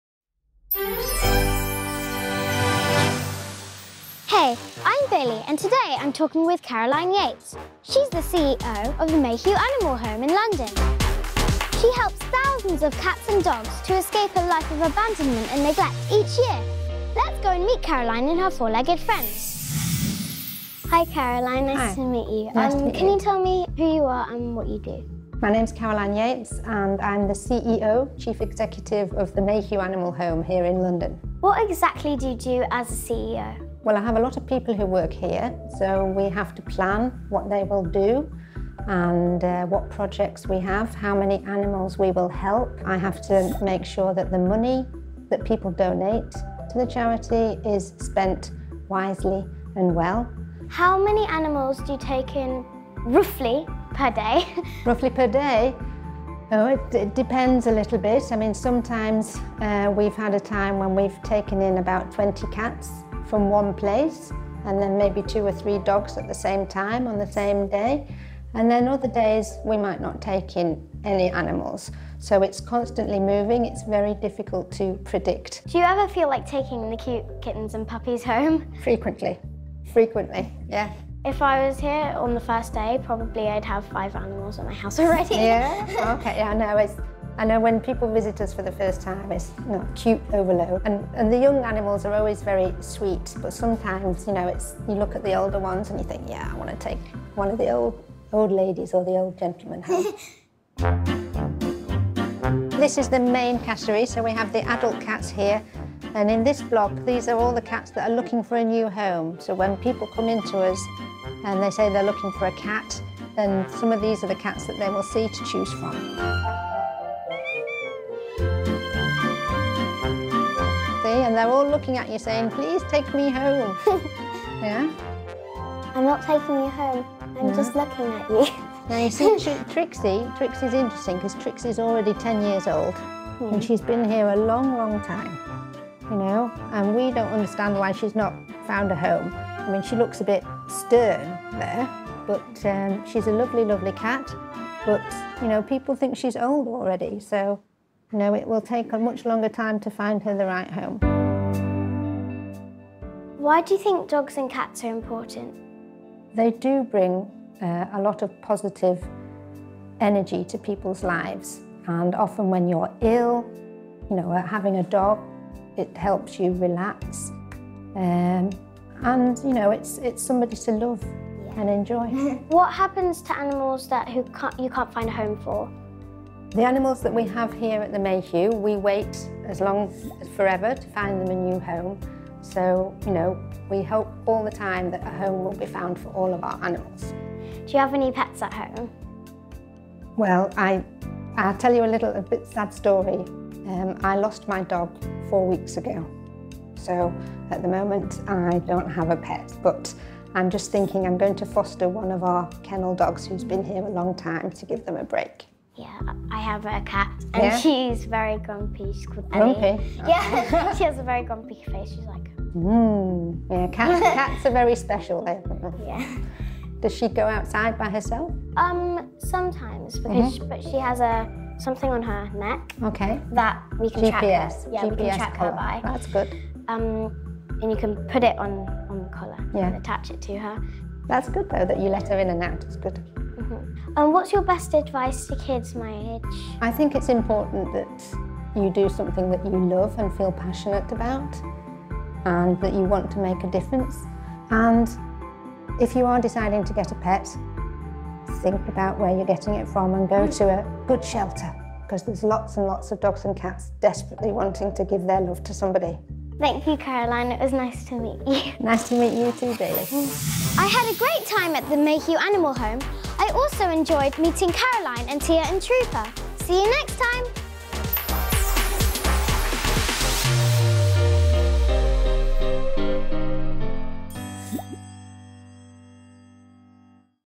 Rescuing Cats and Dogs! | Caroline Yates | Kidspiration
An interview about rescuing cats and dogs with Caroline Yates, the head of The Mayhew Animal Home, London.

Where kids interview the world's most amazing people.